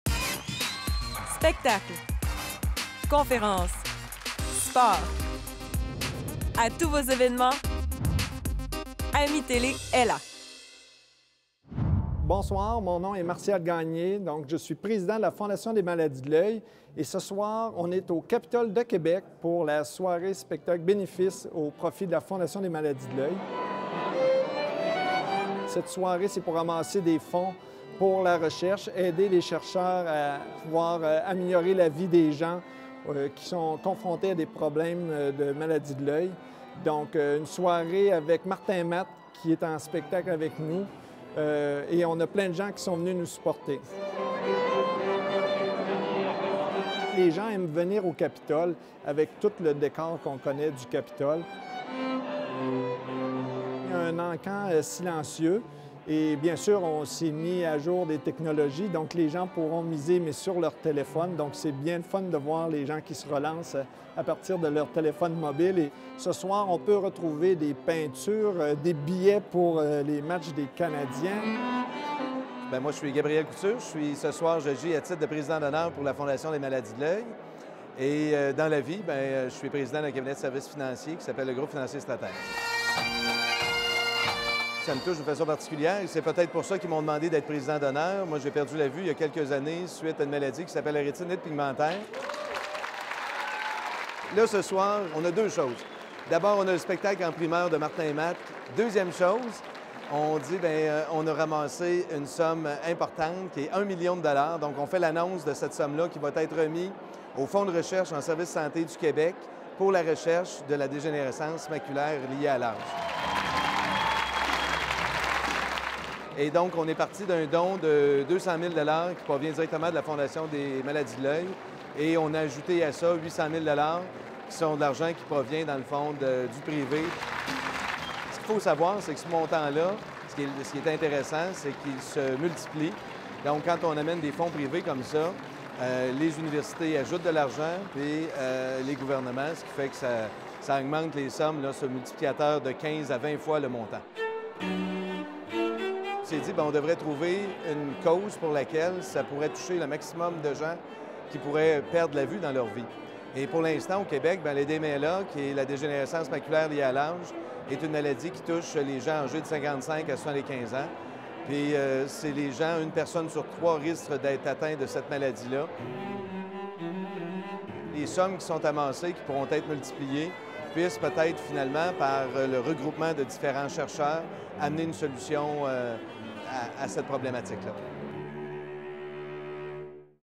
Soirée-spectacle de la Fondation des maladies de l'oeil
Gabriel Couture, président d'honneur de la Fondation des maladies de l'oeil, a fait l'annonce d'un don de 1 million de dollars lors de la soirée-spectacle au Capitole de Québec mettant en vedette Martin Matte.